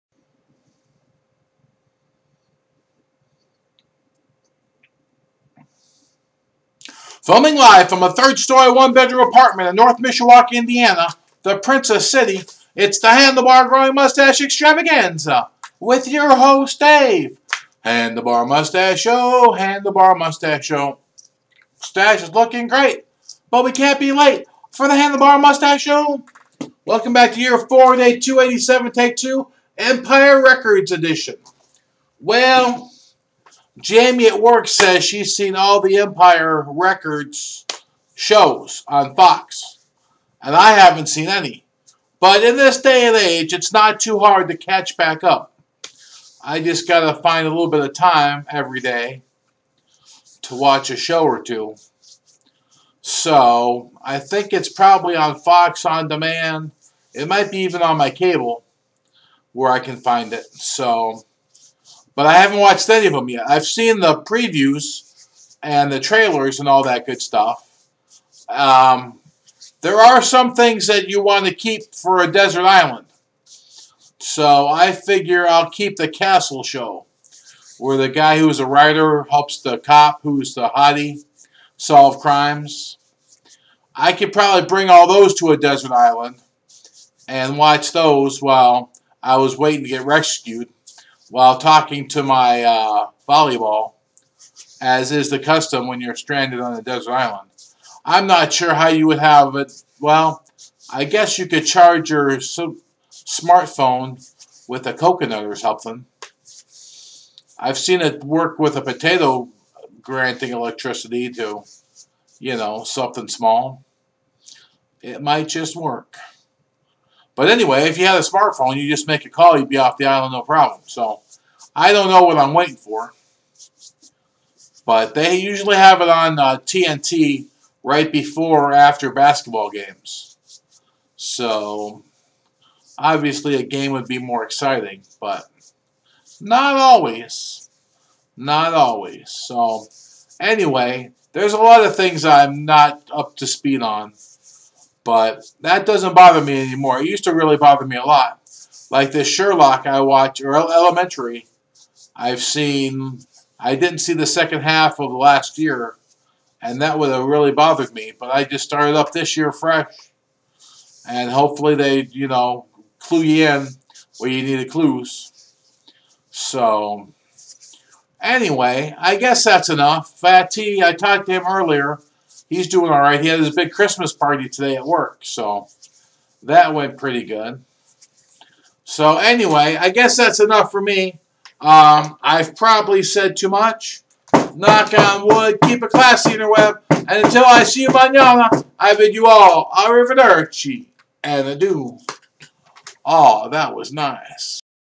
handlebar...year 4 day 287 take 2 Empire Records edition
Dave talks about being backed up on his media thanks to sports